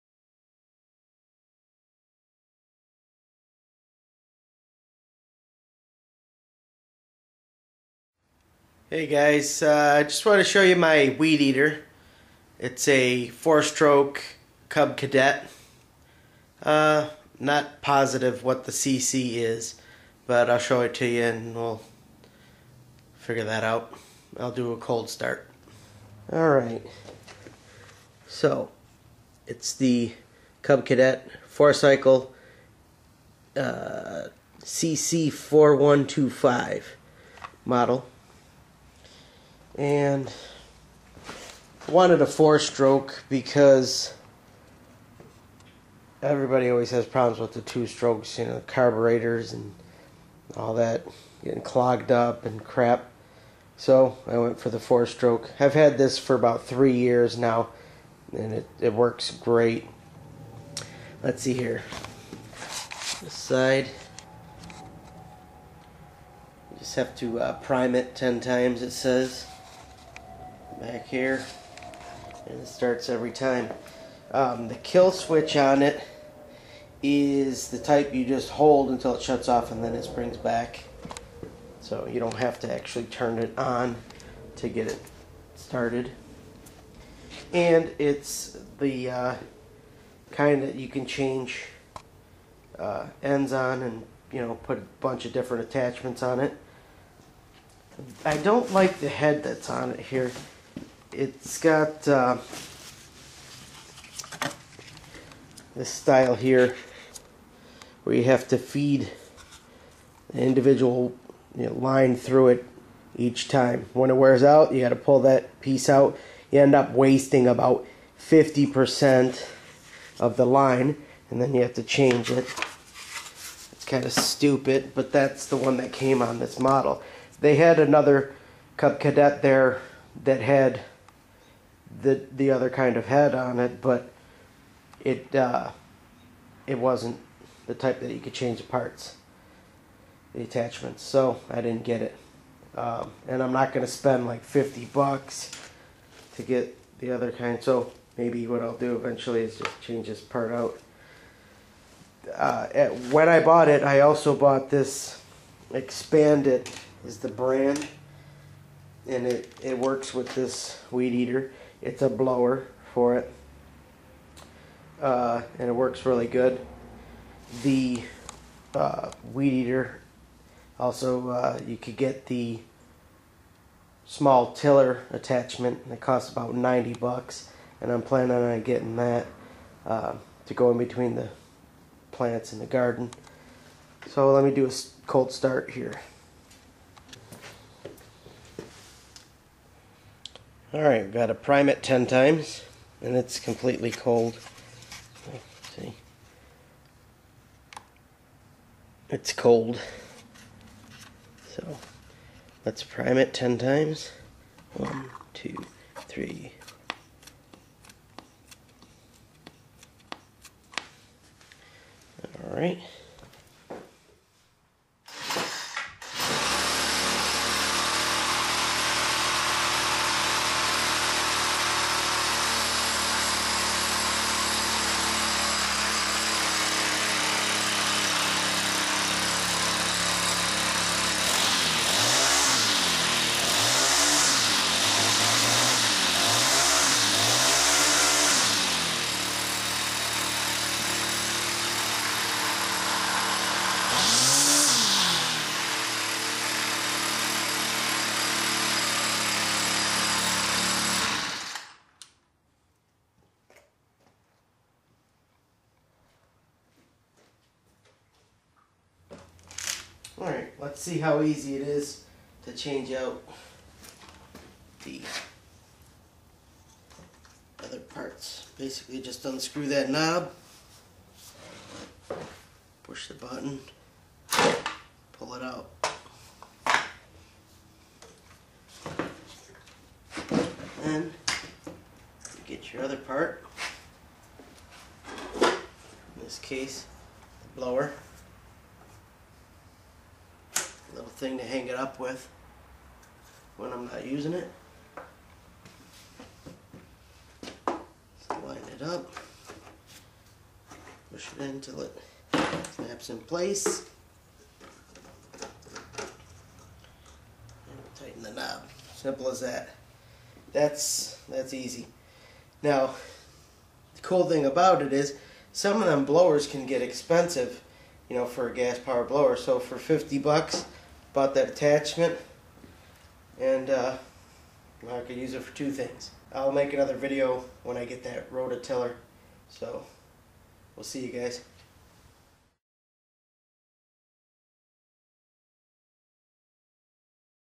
4 Stroke Cub Cadet CC 4125 String Trimmer
This is my Cub Cadet 4 stroke weed eater or string trimmer.  It is model CC4125 and has the ability to add attachments.  I have the blower attachment.  I do a cold start too.